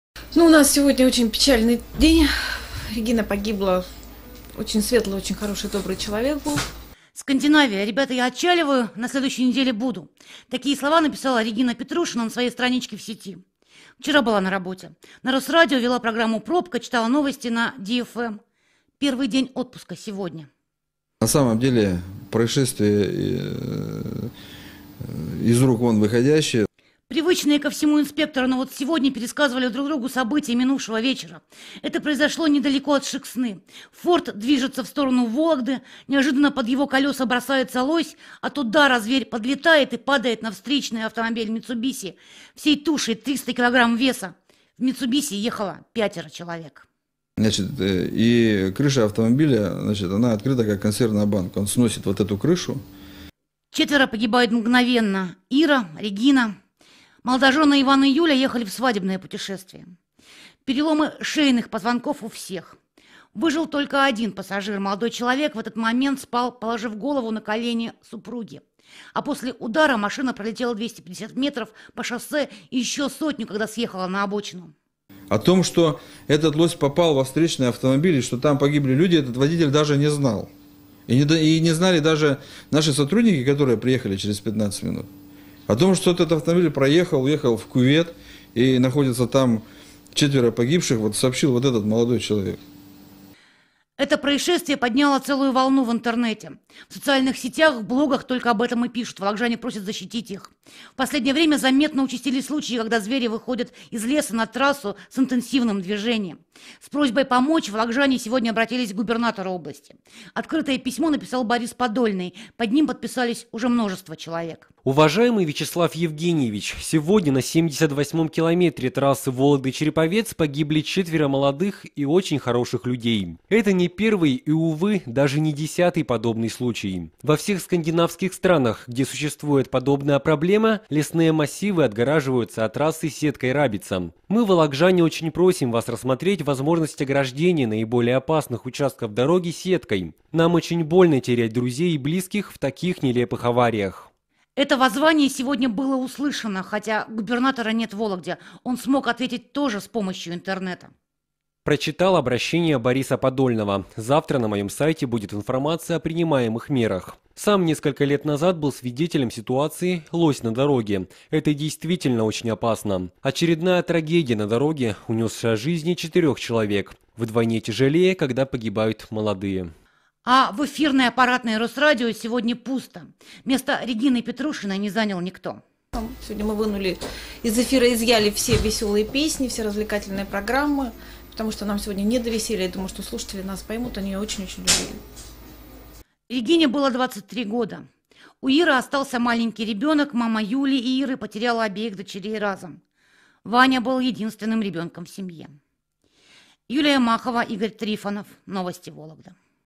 Столкновение с лосем. Человеческие жертвы. Вологда. 2011
Столкновение произошло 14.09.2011 на 78-м километре трассы Вологда – Череповец.
Четыре человека из пятерых находившихся в салоне погибли.

Дополнительная информация на странице “Охотоведение”:
- в Фейсбуке –